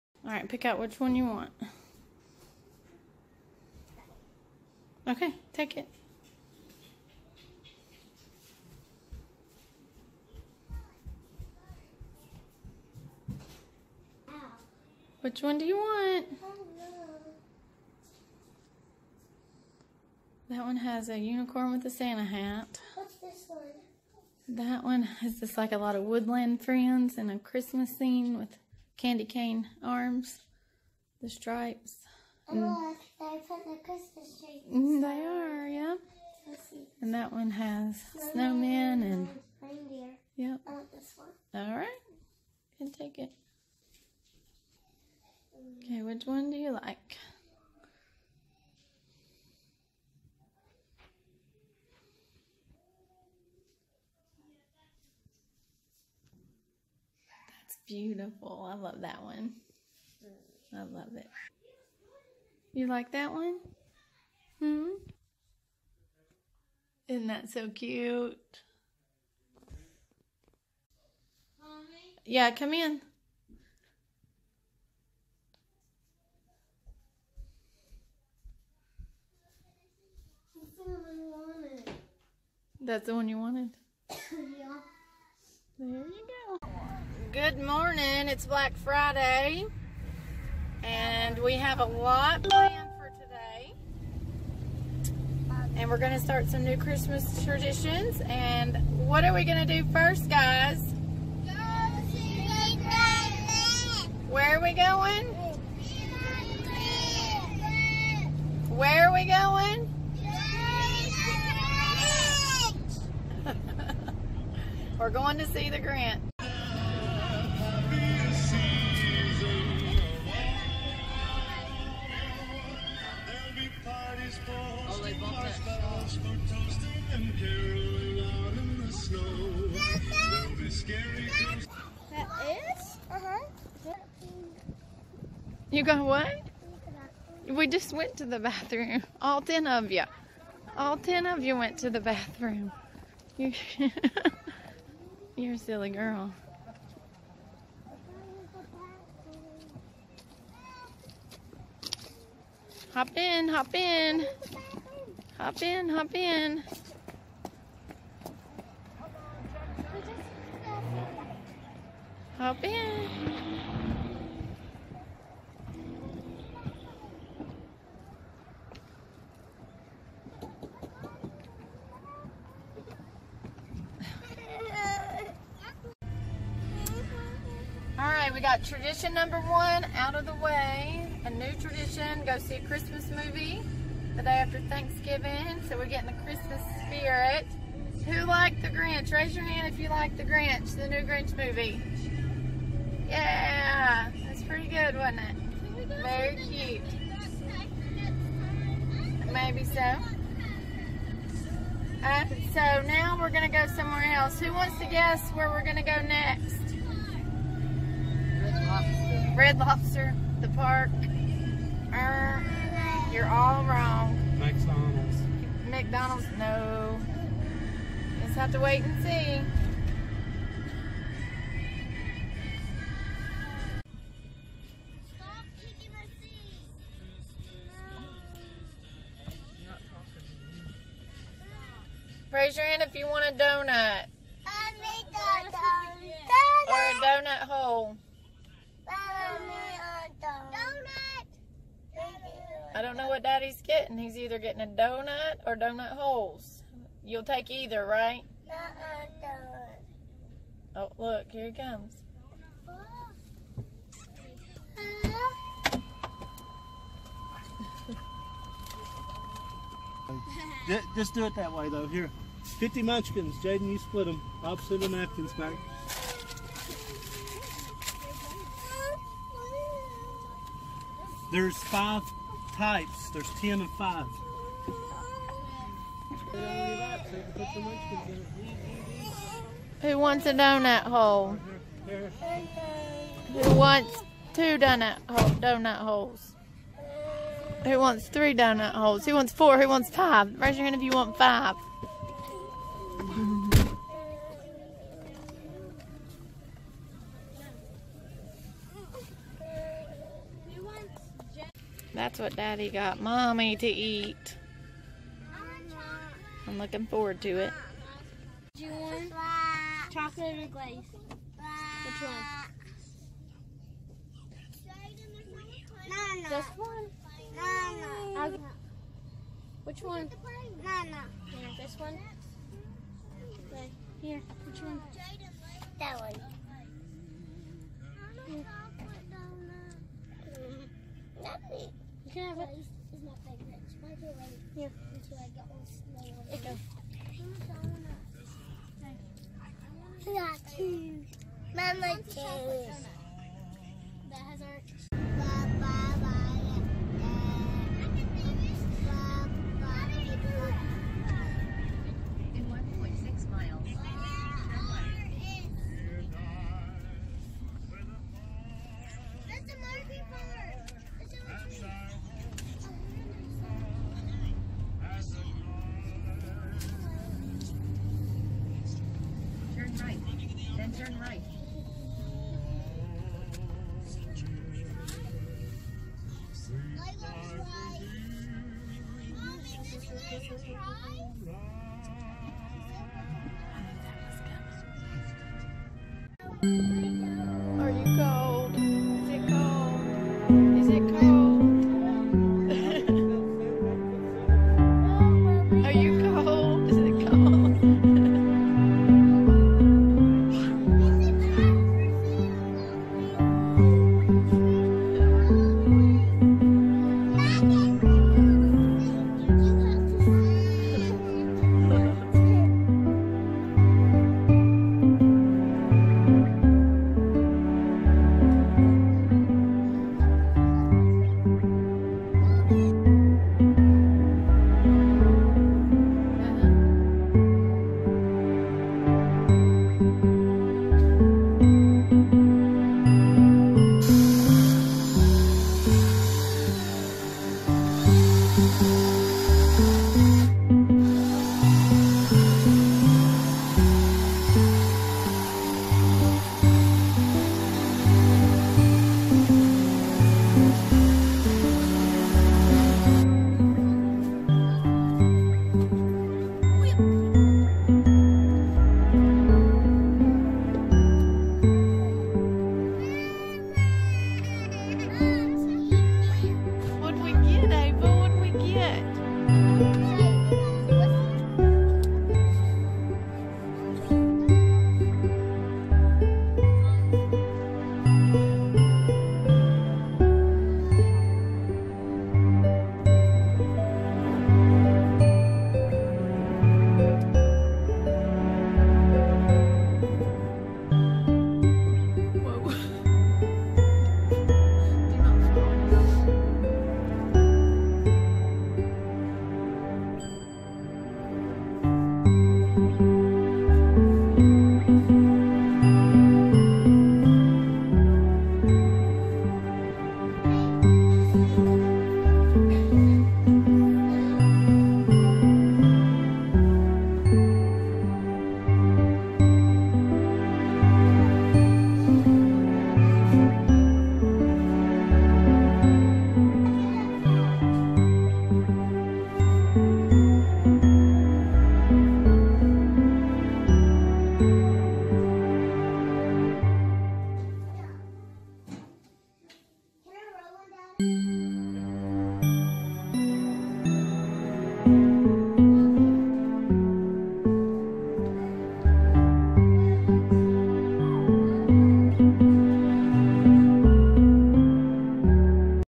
Let the Christmas Traditions Begin!
What Christmas traditions does your family have?

We are adding a few more traditions to our celebrations this year! We kicked off the Christmas season by watching a Christmas movie and picking out a tree at a farm.